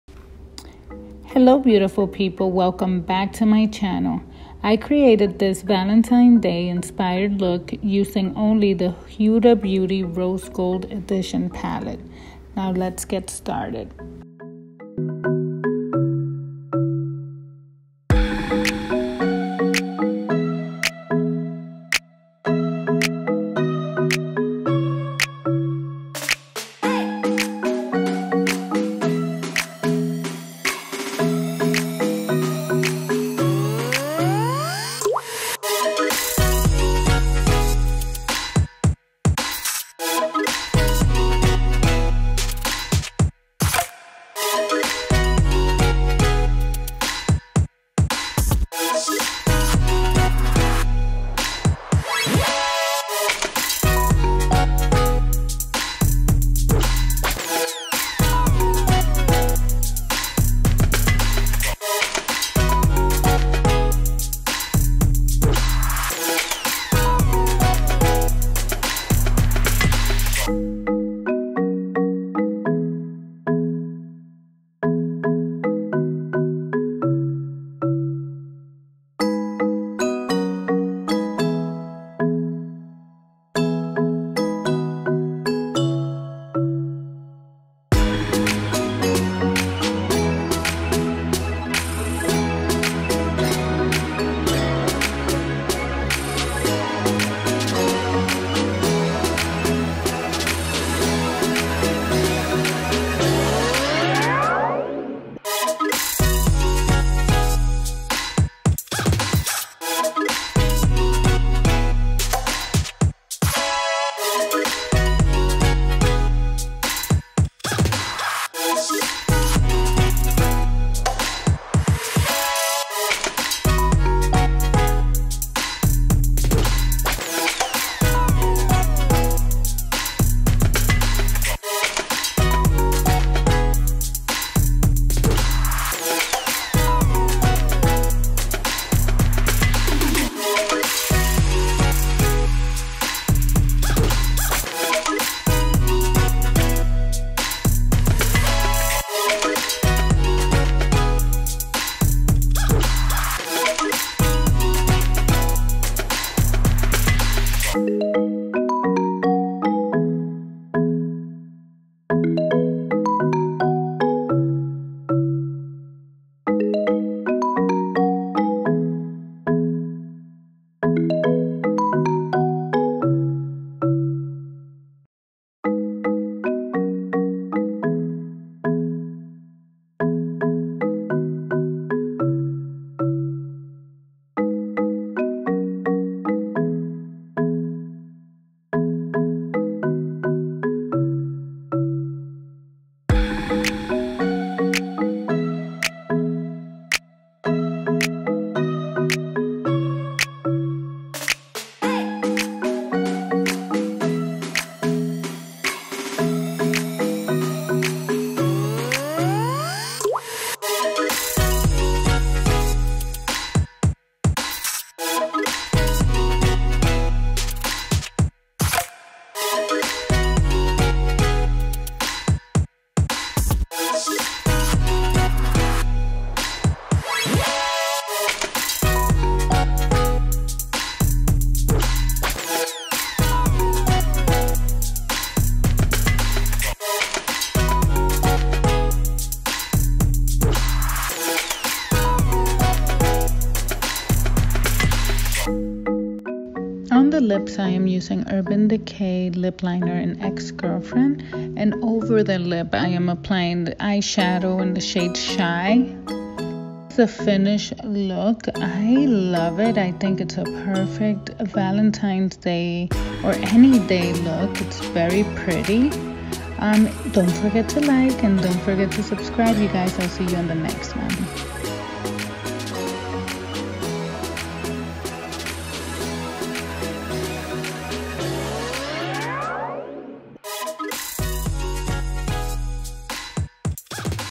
Valentines Day Inspired Look Using Only The Huda Beauty Rose Gold Eyeshadow Palette
Hello Beautiful People! To make it more challenging I decide to use all the colors in the Huda Beauty Rose Gold Edition Palette.

The face portion of the tutorial went missing check out my blog for a complete list of products used and more!